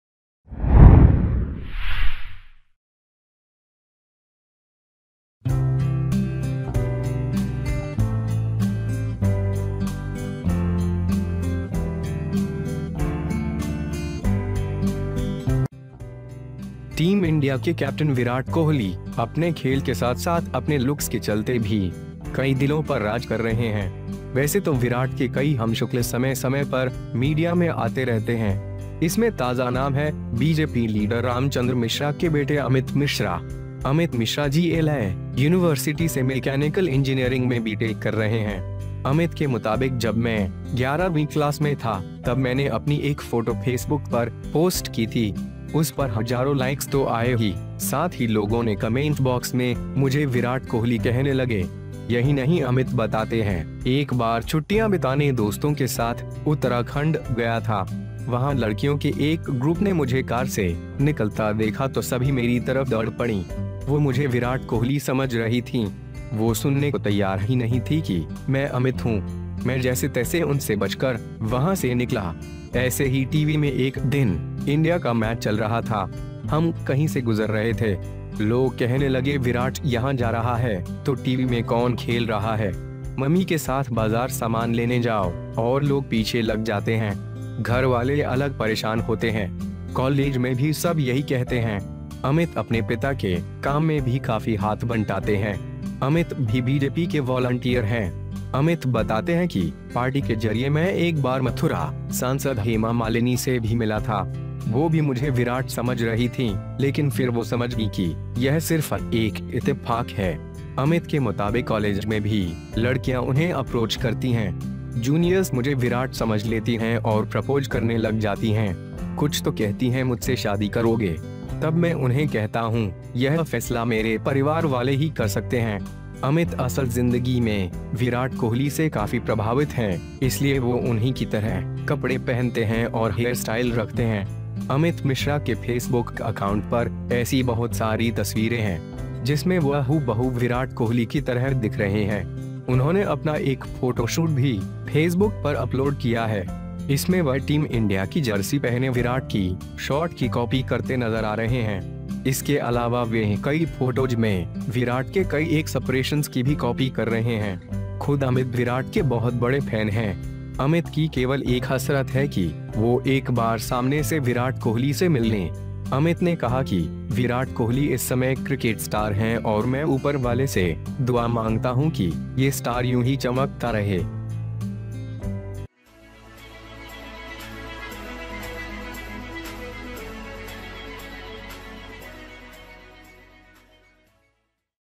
बीजेपी नेता का बेटा है विराट का हमशक्ल, रोजाना आते हैं शादी के प्रपोजल
Team India captain Virat Kohli has ruled many hearts with his game as well as his looks. By the way, many of Vishal's times have come in the media from time to time. Amit Mishra, son of BJP Leader Ramchandra Mishra, is the latest name in it.

Amit Mishra is doing B.Tech in Mechanical Engineering from GLA University. According to Amit, when I was in the 11th class, I posted my photo on Facebook. There were thousands of likes on it, and people started calling me Virat Kohli in the comment box.

Not only this, Amit says, "Once upon a vacation with friends, went to Uttarakhand, where a group of girls saw me coming out of the car, then everyone ran towards me, he was thinking of Virat Kohli, he was not ready to listen. It was that I am Amit. I rescued him from the way and came out of there.

One day India was on a similar TV show, we were passing somewhere. People started saying, Virat is going here, who is playing in the TV. Take the market accessories with Mummy and people get back. The house has different disturbances. This is what everyone says in college too.

Amit also hands a lot in the work of his father. Amit is also the BJP's volunteer. Amit explains, "Through the party, I met Mathura MP Hema Malini once, she was also considered to be a widower, but then she understood that it is only a coincidence."

According to Amit, girls also approach them in college. Juniors understand me as rich and start proposing. Some say, marry me. Then I tell them this decision can only be made by my family members.

Amit is really influenced by Virat Kohli in life, so he wears clothes like this and keeps his hair style. There are so many pictures on Amit Mishra's Facebook account in which he looks like Hoo Bahu Virat Kohli.

He has also ed a photo shoot on Facebook. In this he is seen copying Virat's shot wearing Team India jersey. Apart from this, he is also copying many of Viraat's many expressions in many photos.

Amit Viraat himself is a big fan of himself. Only one person with Amit has a chance to meet Virat Kohli at the front. Amit said that Virat Kohli is a cricket star at the moment and I beg the darling of the above, that the stars are shining.